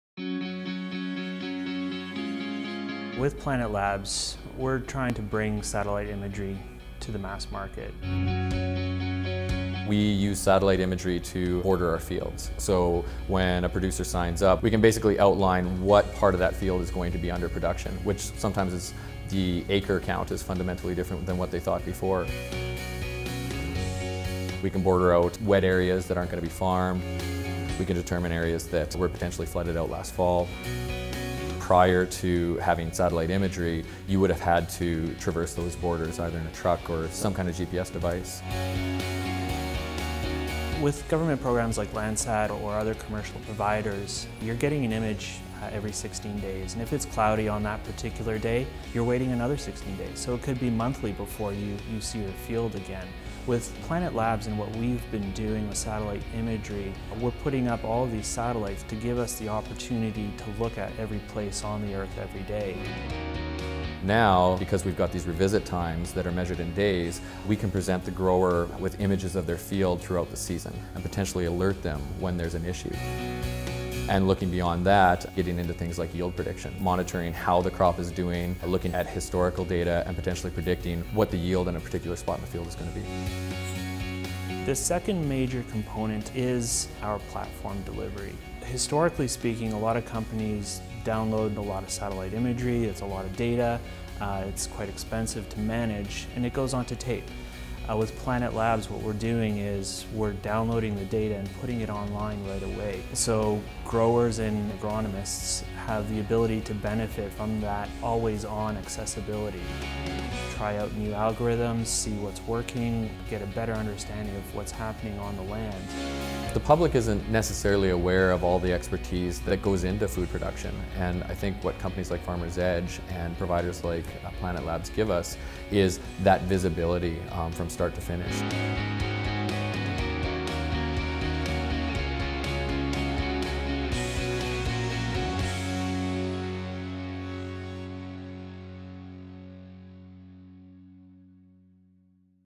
Powering Precision Agriculture: Planet Labs & Farmers Edge
Satellite imagery helps  growers make smarter decisions about their crops and their land.